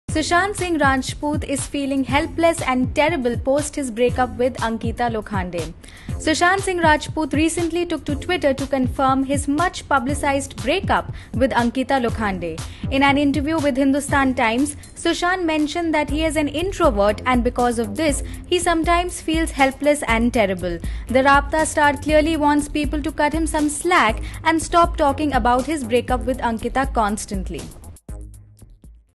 Sushant Singh Rajput is feeling helpless and TERRIBLE post his break with Ankita Lokhande!
Sushant Singh Rajput recently took to Twitter to confirm his much publicised break up with Ankita Lokhande. He also seemed visibly disturbed as he denied all rumours connecting to their ugly fights, his philandering and her alcoholism! In his fiery tweet, he said that the only reason they broke up was because they grew apart.

Well, both SSR and Ankita have been sending out cryptic messages to each other via social media sites leaving their fans confused and hopeful at the same time. But Sushant definitely gave a lot of clarity with this tweet and seems to have been nursing his break up since then. The actor who is now shooting for Raabta with Kriti Sanon was also in the news for getting close with her.

Hindustan Times spoke to this talented actor about these ongoing controversies about his personal life and he said, “If you are discussed, scrutinised and judged in public not as an actor but for your personal life, you can’t complain. However, you sometimes feel helpless and terrible. Especially if you are an introvert.”

The Raabta star clearly wants people to cut him some slack and  stop talking about his break up with Ankita constantly.